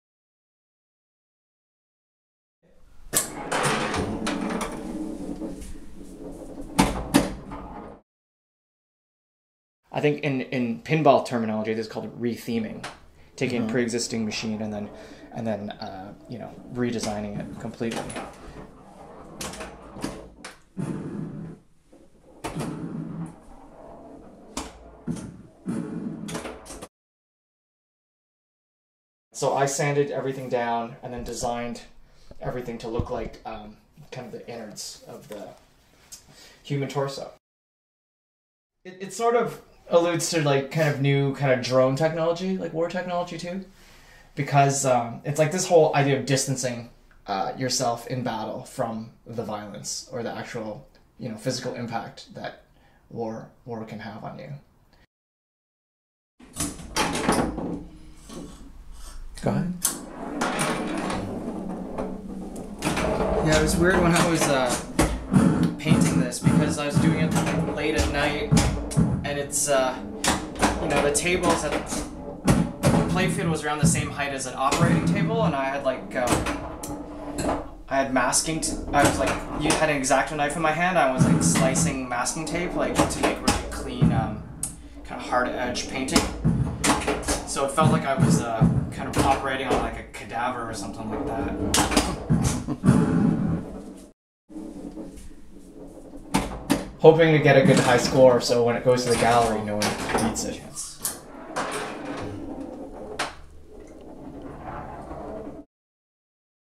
Big Beat video: Ottawa artist Howie Tsui
Ottawa artist Howie Tsui plays his new pinball game Musketball!, created for his new exhibition on the War of 1812.